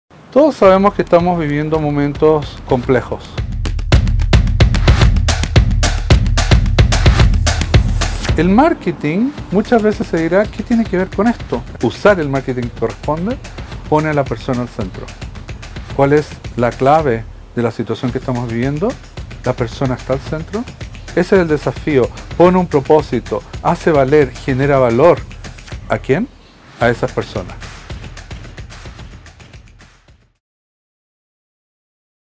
Magíster y Diplomado en Dirección de Marketing | Escuela de Negocios UAI (Cápsula)
Bracey Wilson, director del Diplomado y Magíster en Dirección de Marketing entrega un tip para abordar, desde el marketing, la situación actual.